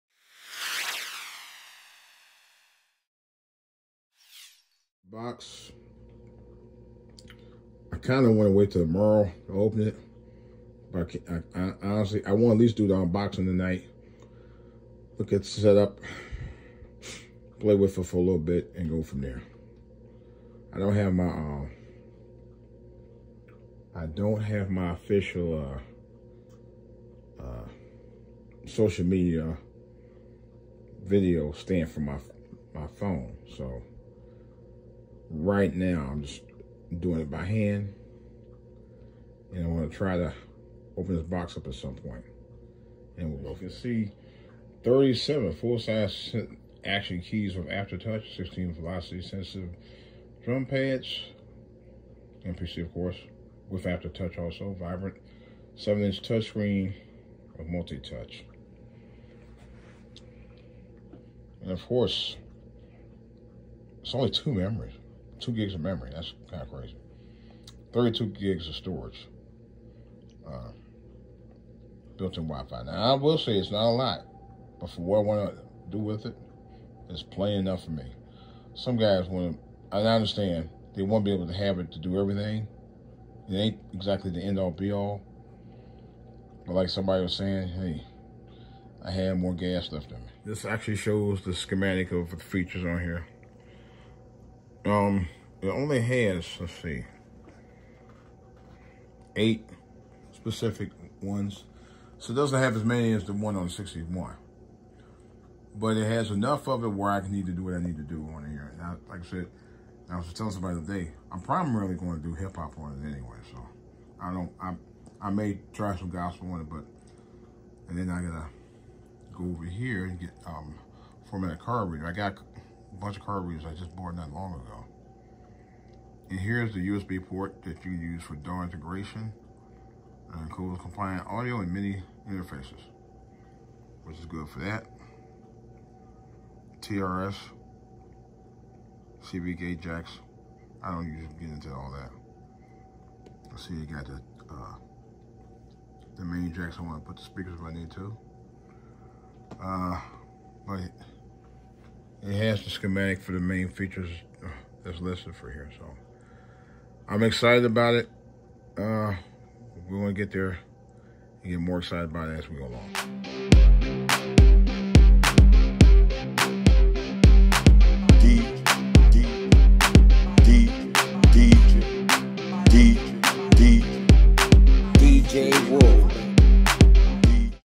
The GAS Show on Akai MPC 37 key!
I played with it last night and I have a need to create just hip hop on it.